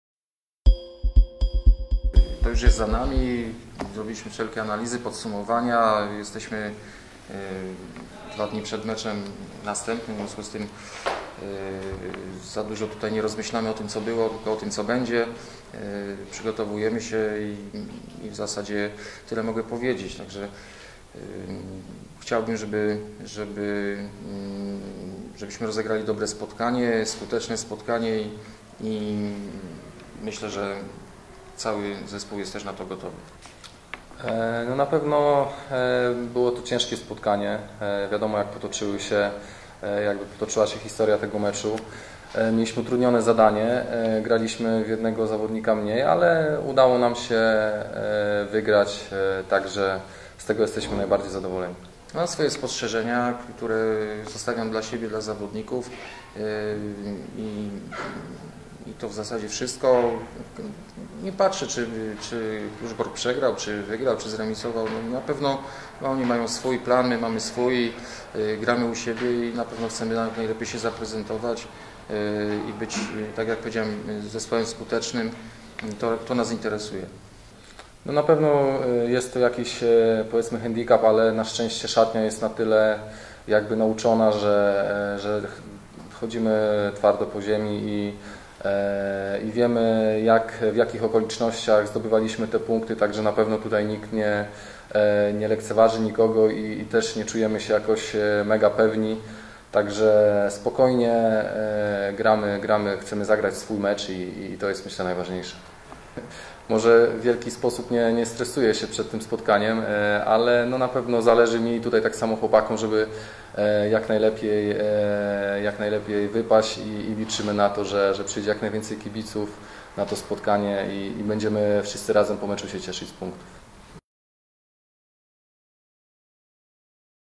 Konferencja przed meczem z MKS-em Kluczbork // Raków News TV
Konferencja przed meczem z MKS-em Kluczbork z udziałem trenera Radosława Mroczkowskiego i Wojciech Okińczyc // Raków News TV